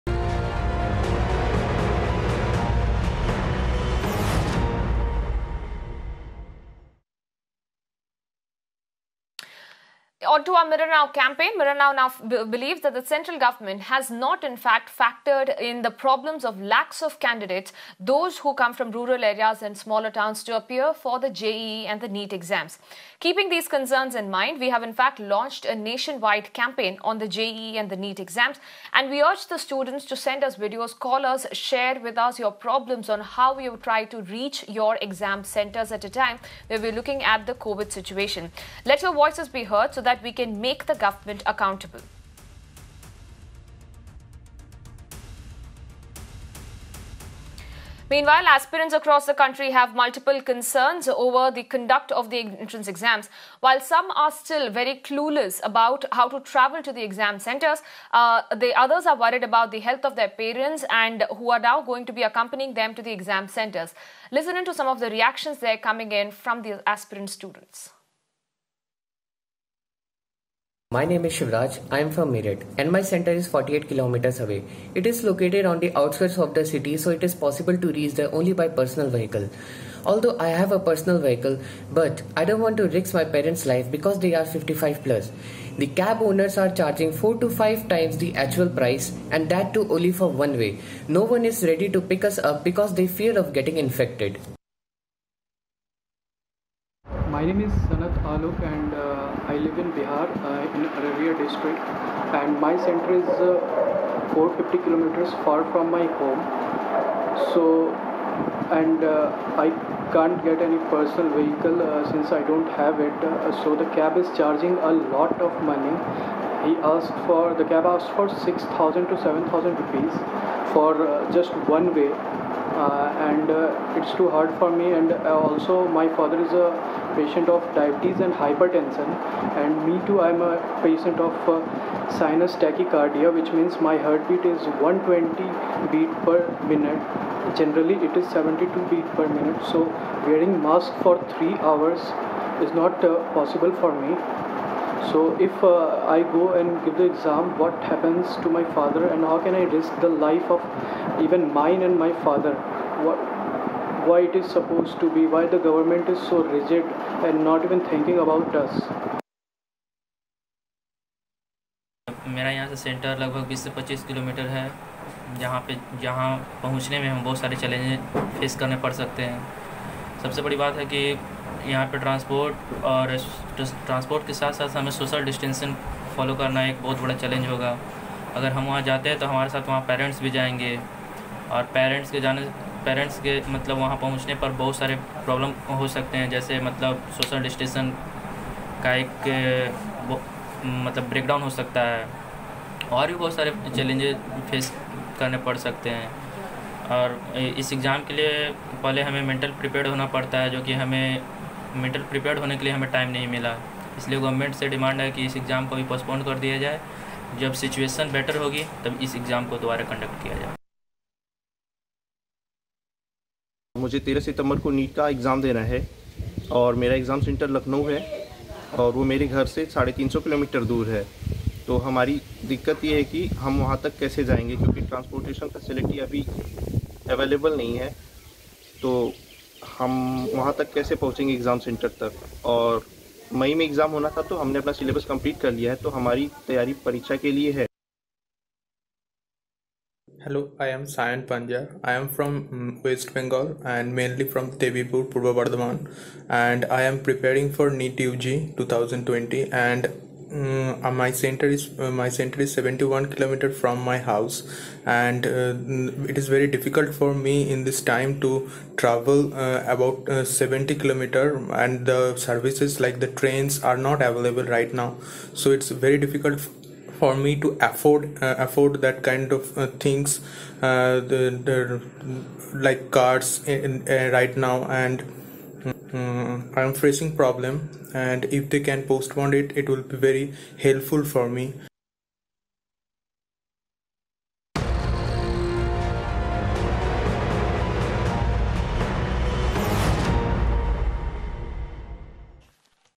NEET aspirant Shivraj shares concern over safety & travel | India With Students
In Mirror Now's exclusive India with Students segment, we ask NEET-JEE aspirants to share their grievances and pose a question to the government. This is our attempt to make the government see the problem and change their decision on the matter of conduction of competitive exams. The NEET-JEE exam is scheduled to take place on September 1.

This decision has caused many protests and condemns from people given the ongoing coronavirus pandemic. Students have raised concerns about their safety. Many students do not even have the means to travel to centres., while many living in remote areas affected by floods will also not be able to make for their exams. Moreover, the students are also worried about their parent's safety who will accompany them for their exams.